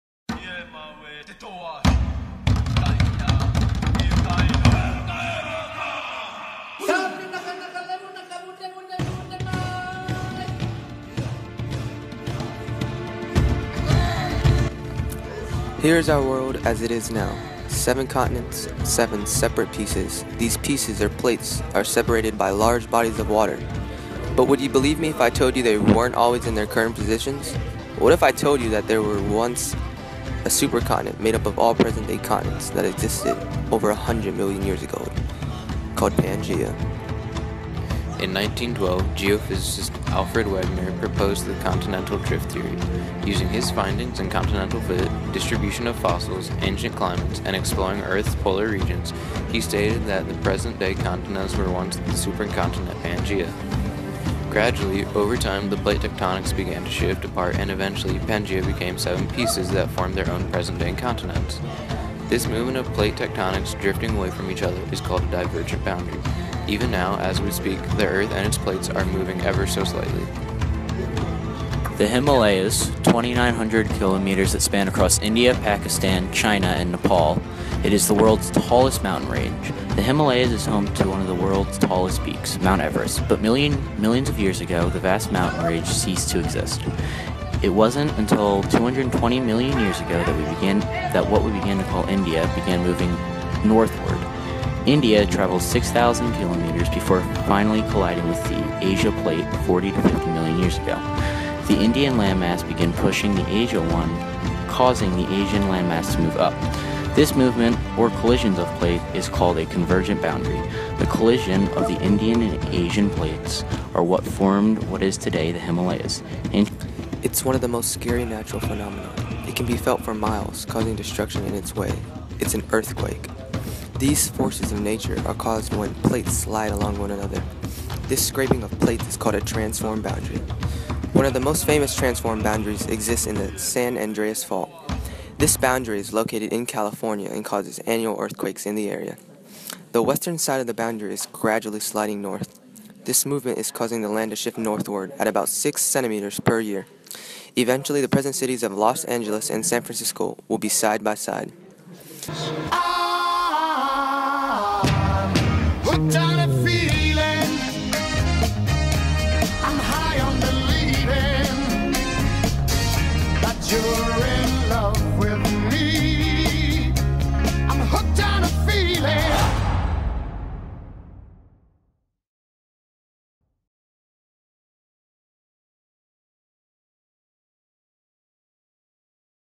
Plate Tectonics Stop Motion
PCHS Sci Viz 2 claymation project. Enjoy ;)